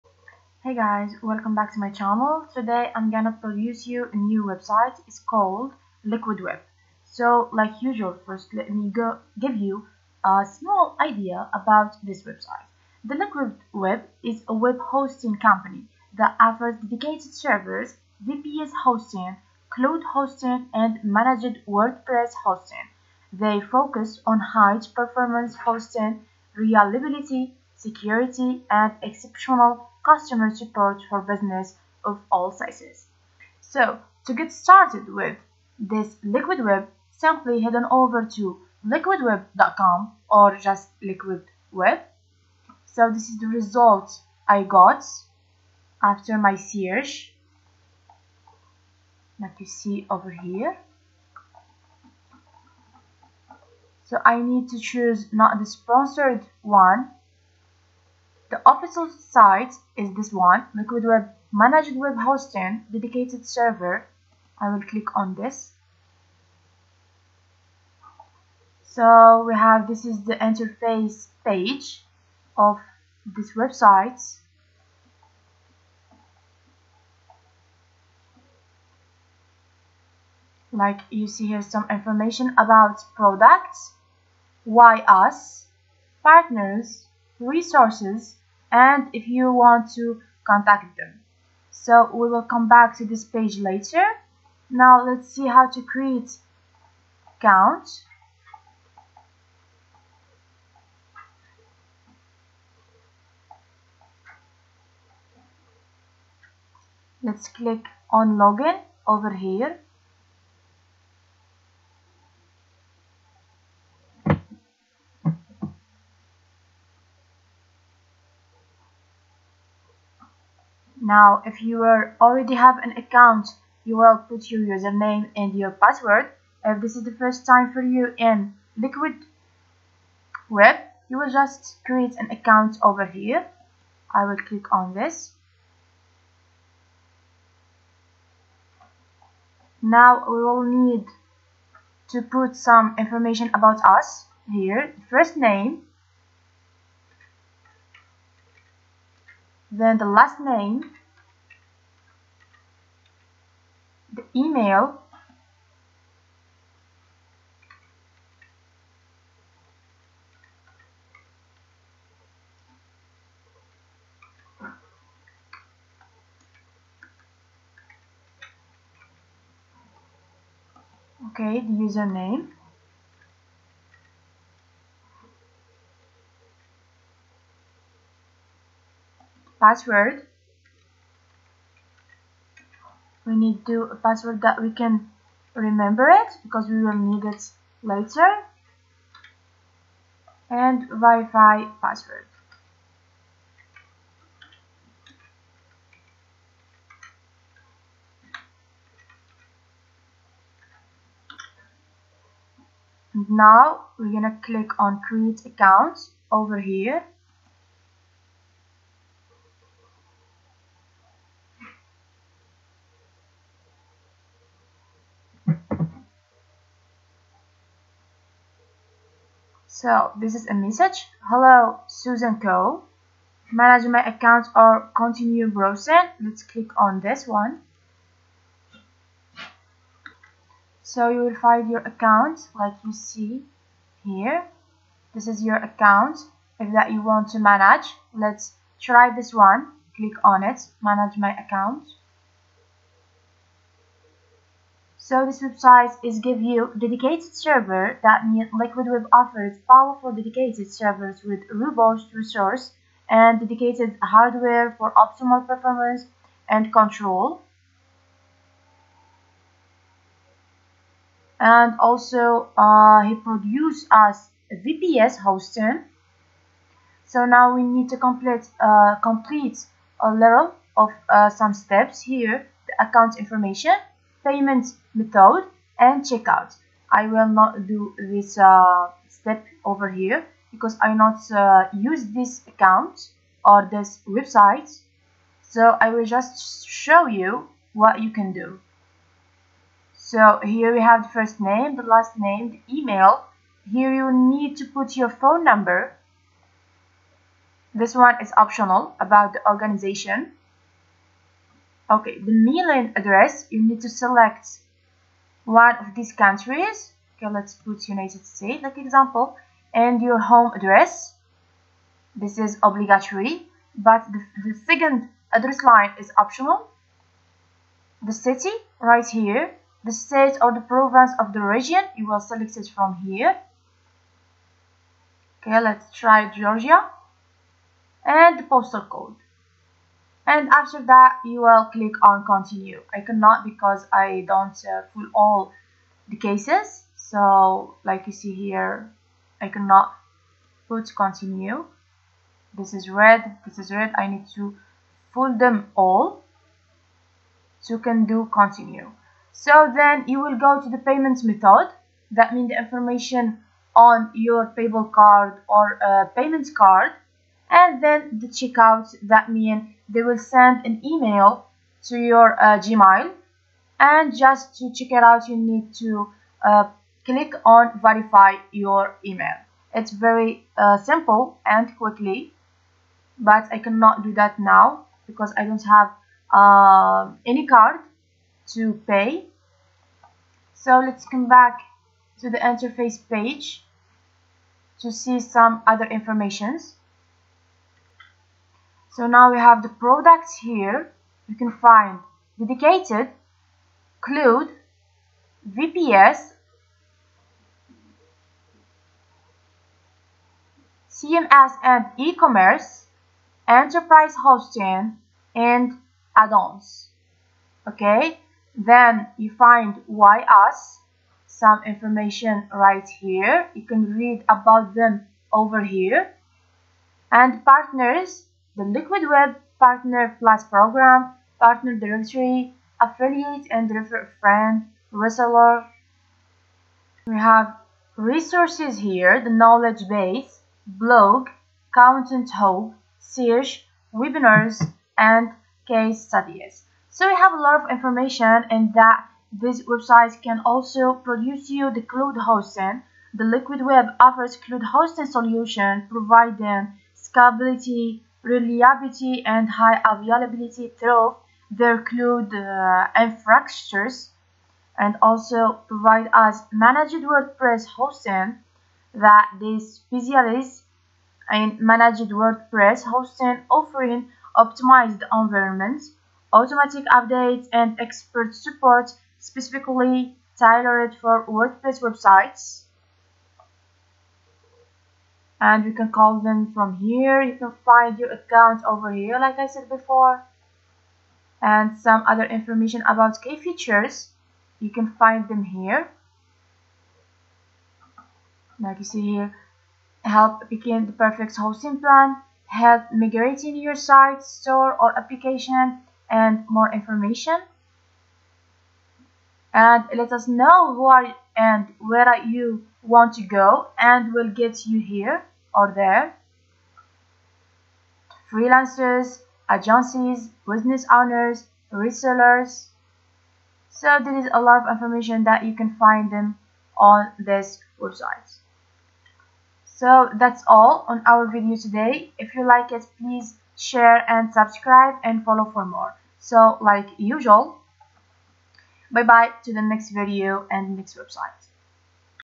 How to Use LiquidWeb: A Comprehensive Guide to Reliable and Managed Web Hosting
How to Use LiquidWeb: A Comprehensive Guide to Reliable and Managed Web Hosting 2023

in this video we discuss :

LiquidWeb is a trusted and managed web hosting provider that offers reliable and high-performance hosting solutions. In this comprehensive guide, learn how to utilize LiquidWeb to establish and manage your online presence. From setting up your account to choosing the right hosting plan, we'll guide you through the process. Discover LiquidWeb's robust features, including dedicated servers, cloud hosting, and managed WordPress solutions. Explore their intuitive control panel, security measures, and exceptional customer support. Whether you're a small business or an enterprise, LiquidWeb provides the tools and resources for a seamless web hosting experience. Start harnessing the power of LiquidWeb's hosting services and elevate your online presence today.